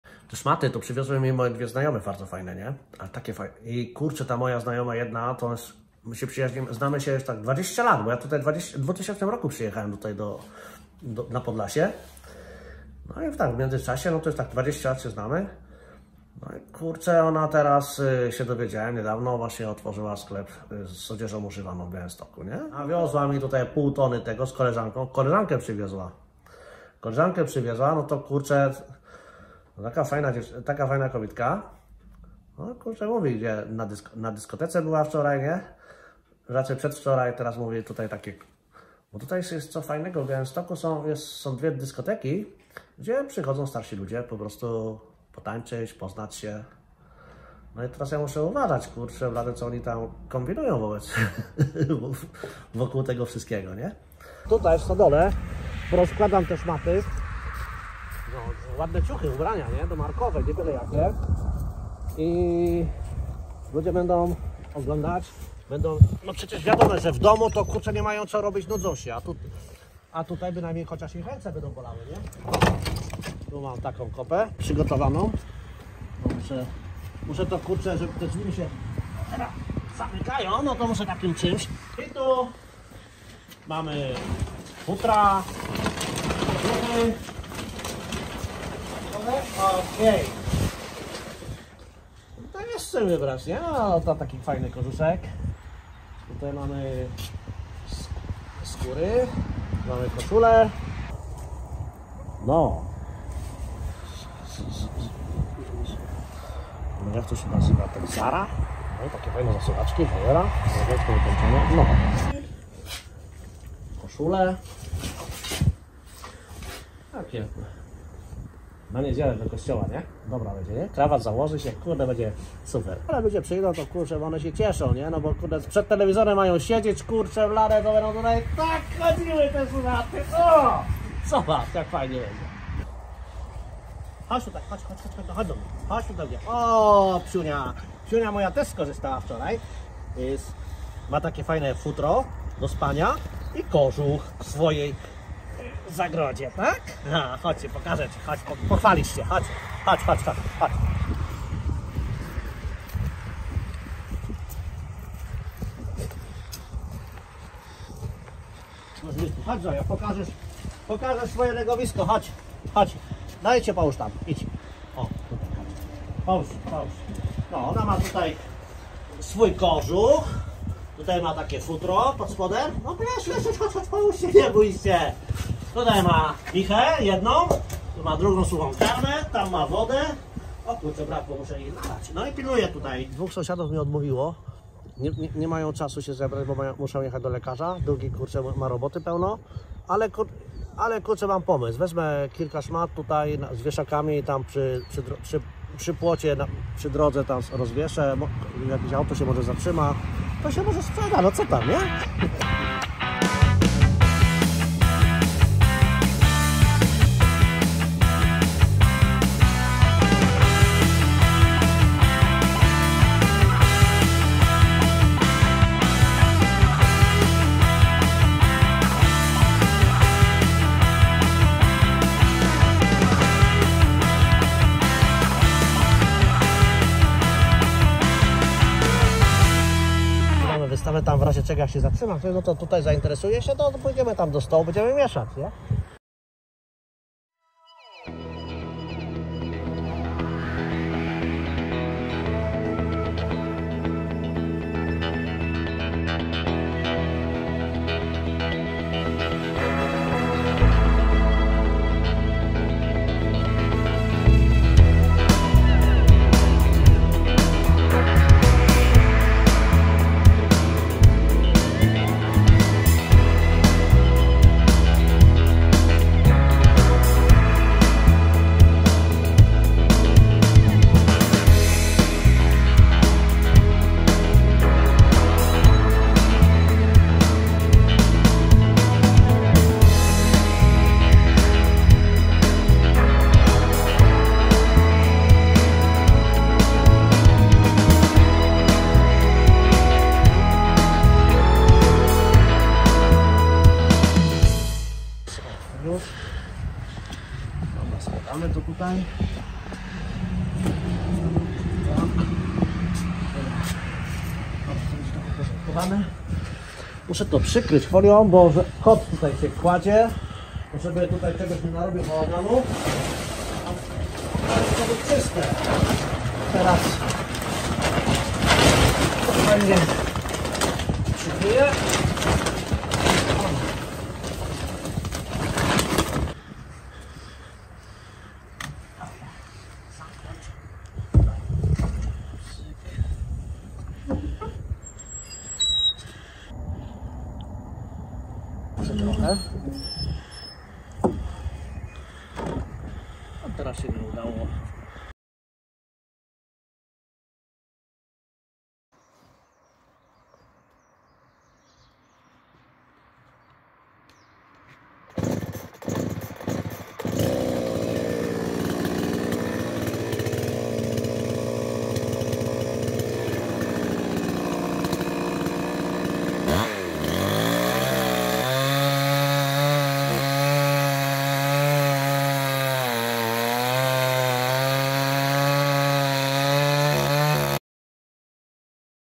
Wiesław Podlasia Król - przed koronacją - biznes
Wiesław długo jeszcze przed koronacją na KRÓLA PODLASIA. Otwiera swój pierwszy biznes.